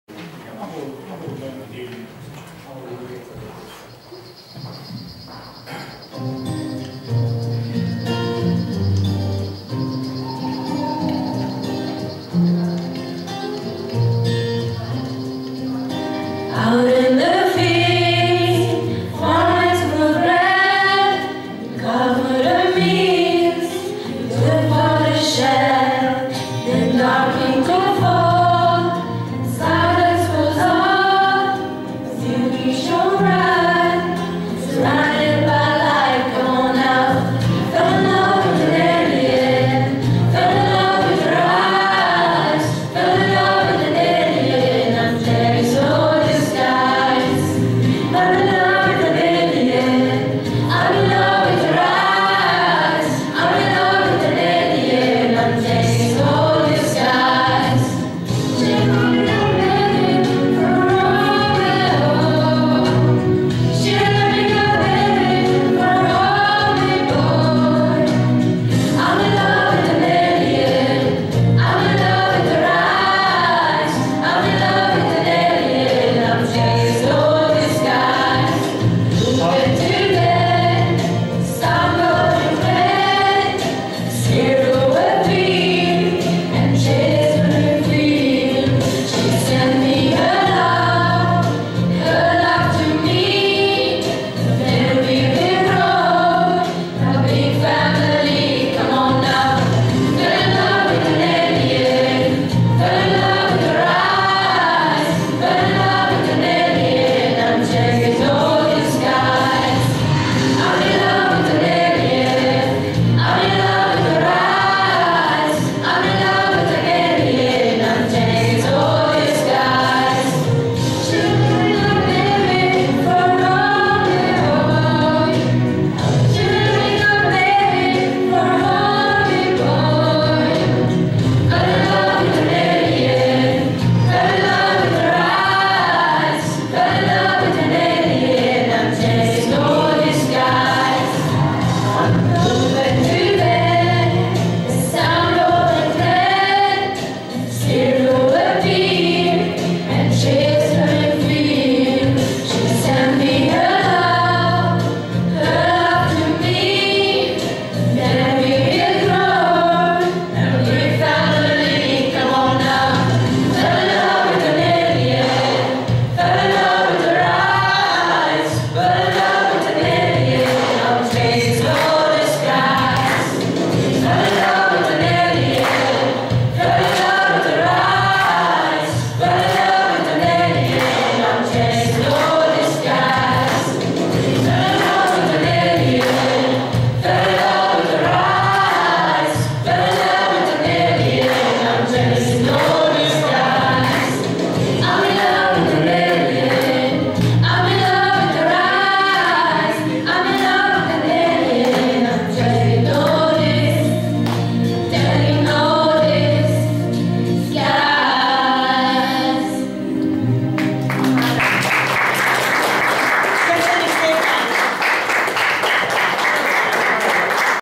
01 An Alien Talent
Nyílvános Talentes próbanap a Zöld Terasz Kulturális Központban Szeged - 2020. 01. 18.